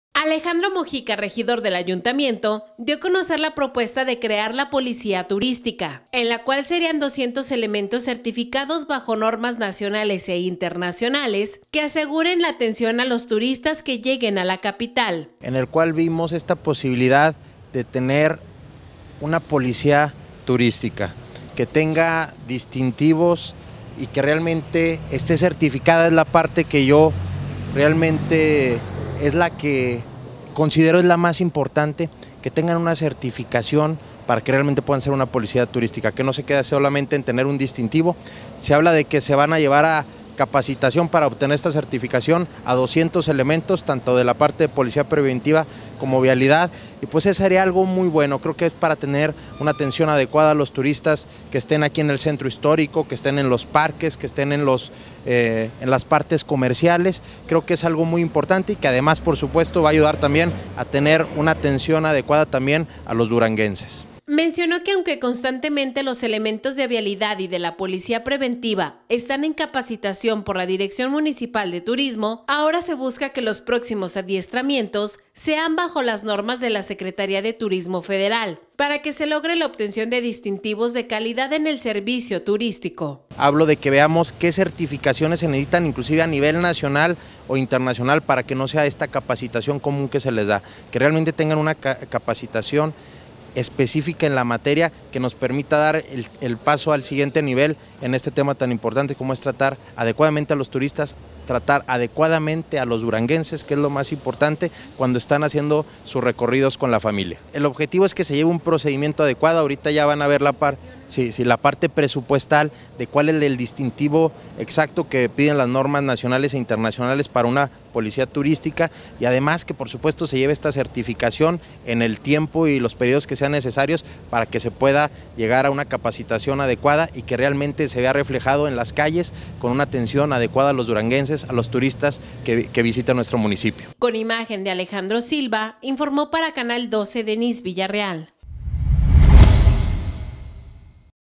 PROPONEN POLICÍA TURÍSTICA /19 FEBRERO 2018
Alejandro Mojica, regidor del ayuntamiento, dio a conocer la propuesta de crear la policía turística. Serian 200 elementos certificados bajo normas nacionales e internacionales que aseguren la atención a los turistas que lleguen a la capital.